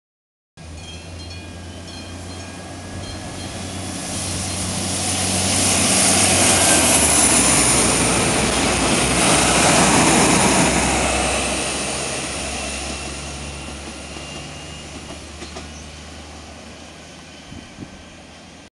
Průjezd vlaku Os 7007 (ČD 844.012) - Málkov, 24. 8. 2013
Vlak zde staví na znamení, tentokrát nezastavil.

Klášterec nad Ohří (13.07) - Most (13.49)
Natočeno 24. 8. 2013 ve 13.19